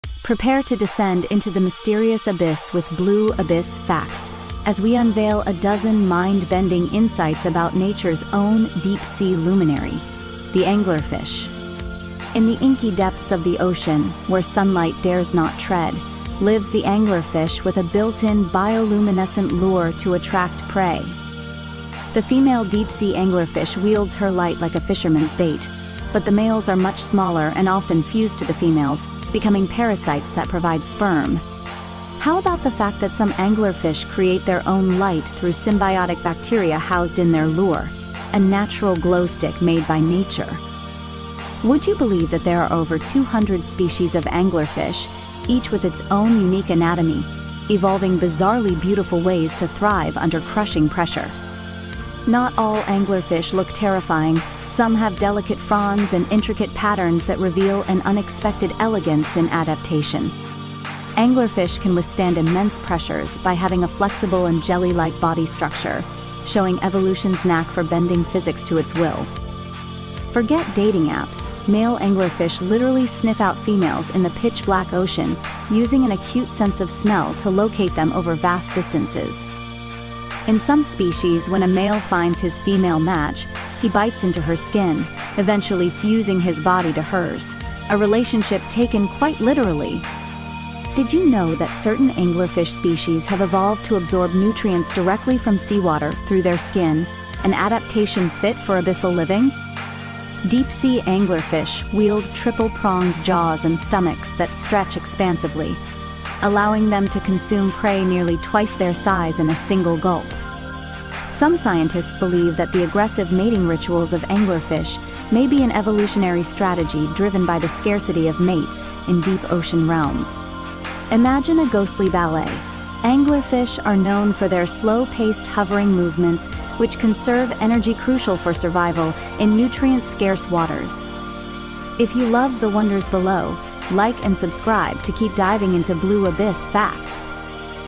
12 Astonishing Deep Sea Angler Facts You Won't Believe!
Dive into the mysterious world of the deep sea with us as we explore the mesmerizing lives of anglerfish! From their glowing lures to jaw-dropping mating rituals, discover 12 incredible facts that reveal their bizarre beauty. Intrigued? Don't miss out—it's a deep-sea adventure you won't forget!